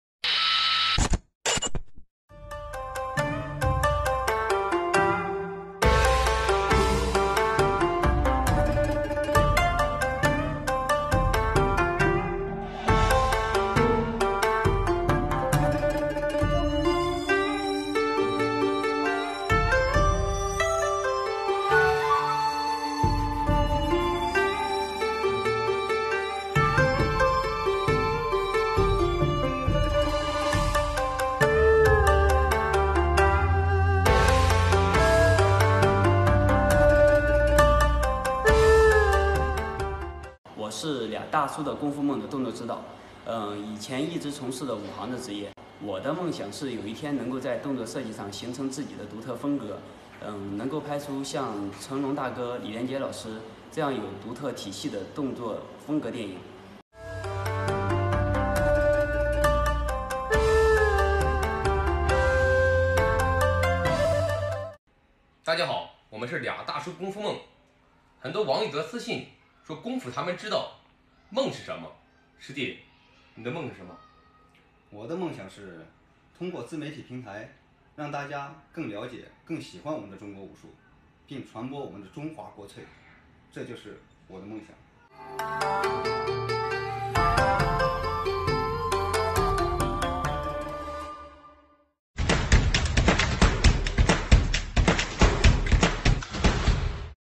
Chinese kung fu stick method
The team of "Two Uncle's Kung Fu Dreams" composed of three young people from Sun Xingang, Wang Qun and Zhang Jikun filmed a martial arts film and became a hit in three days. Sun Xingang said that a martial arts action has been shot more than 40 times, although it will be injured but not serious. The homemade martial arts films were completed in May, and they were made by themselves in the early and late stages.